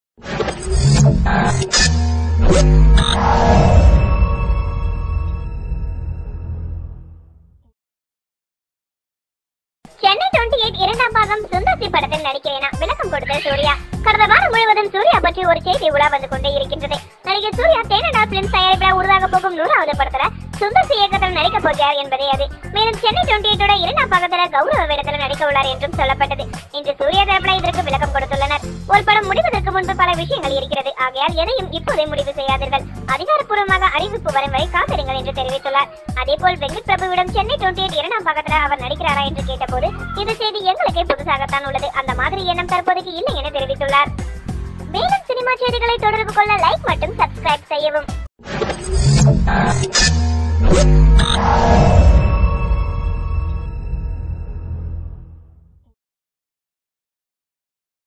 சென்னை 28 2ம் பாகம், சுந்தர் சி படத்தில் நடிக்கிறேனா விளக்கம் கொடுத்த சூர்யா|TamilUlagam
TamilUlagam Latest Cinema News